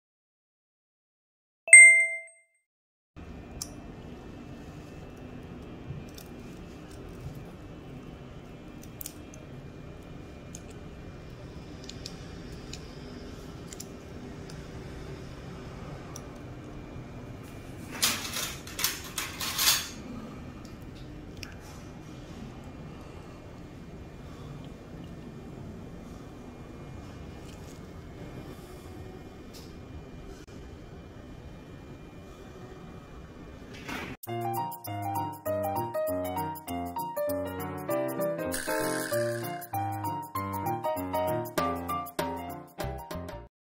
[ASMR] Severely Broken, Decayed Wisdom Tooth Extraction
Welcome!🦷

Tooth decay left untreated for a long time is often impossible to treat, so it is recommended to have regular dental checkups.